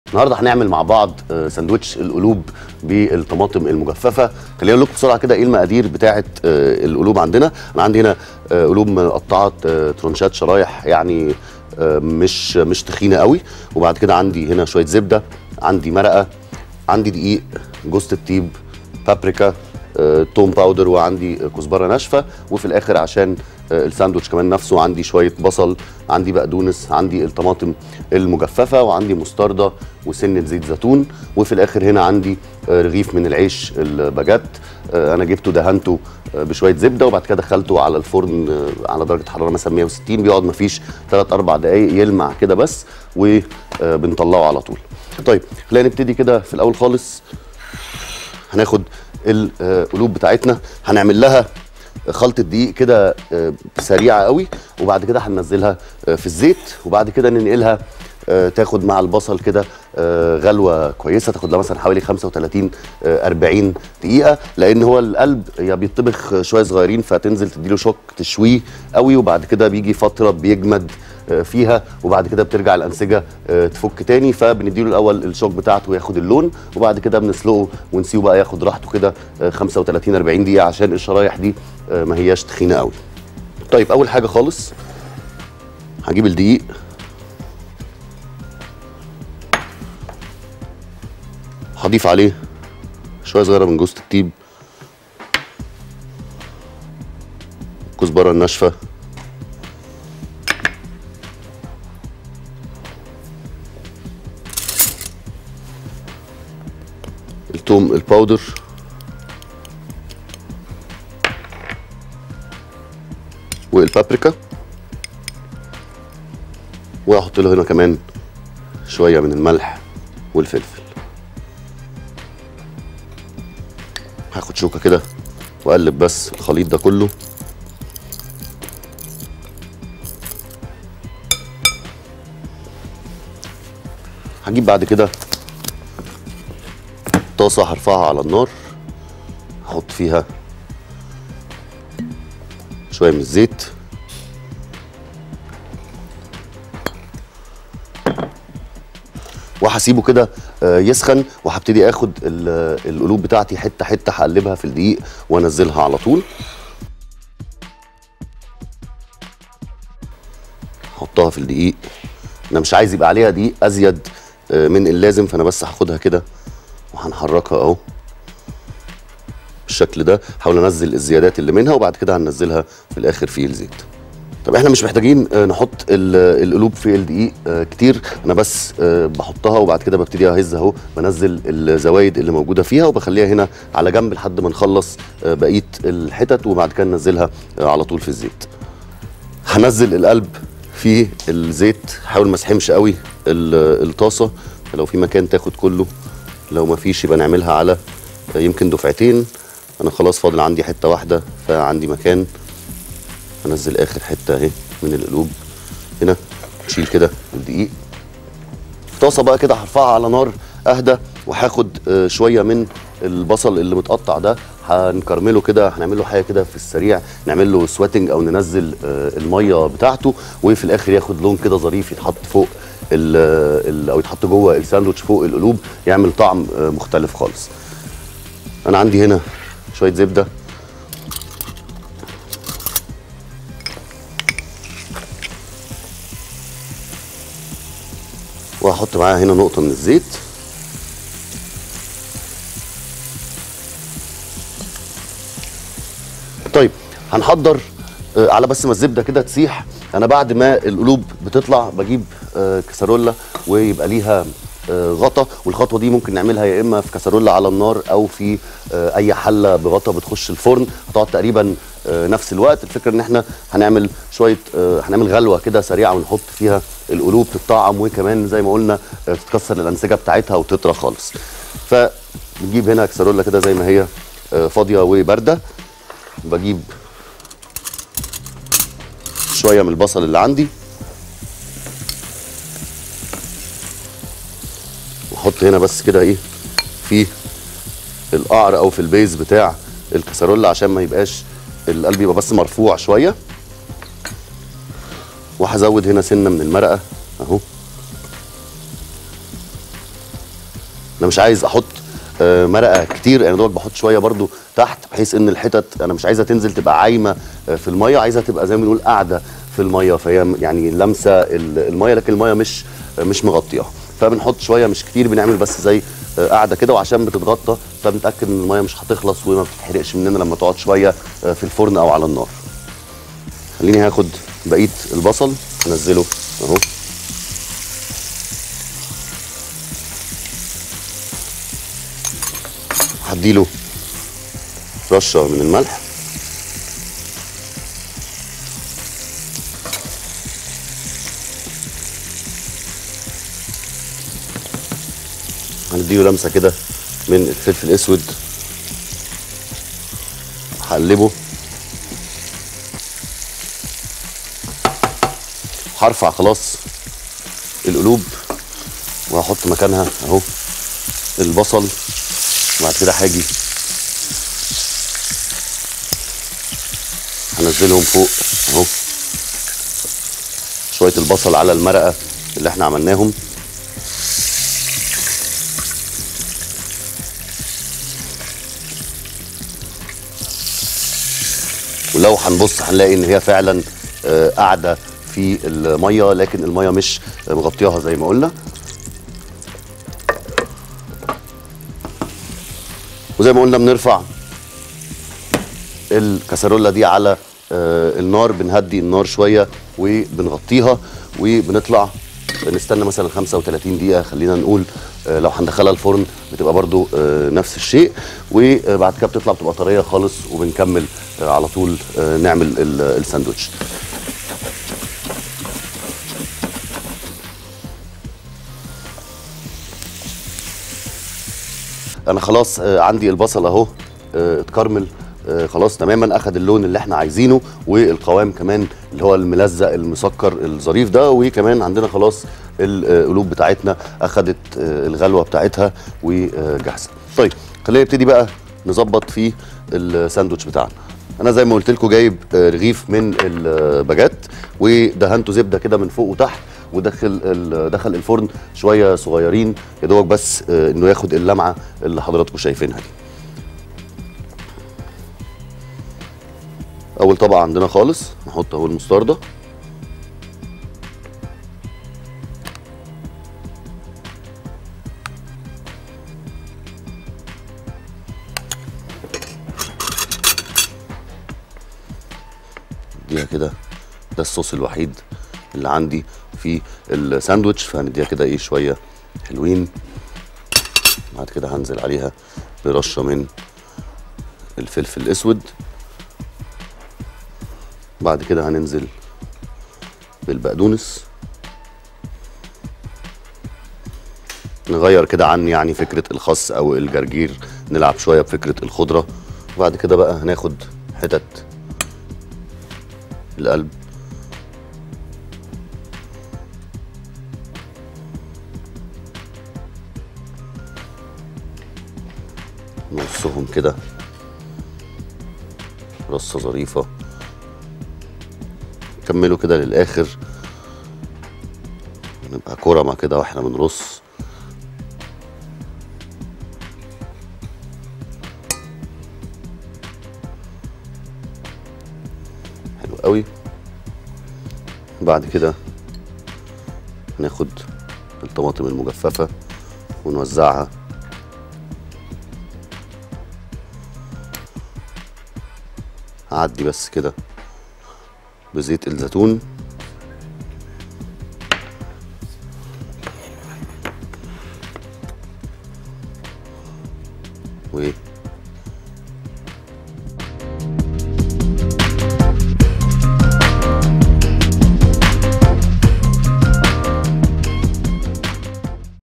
سندوتش قلب بقري | عمر اسماعيل
طريقة عمل سندوتش قلب بقري مع الشيف عمر اسماعيل في  نأنأة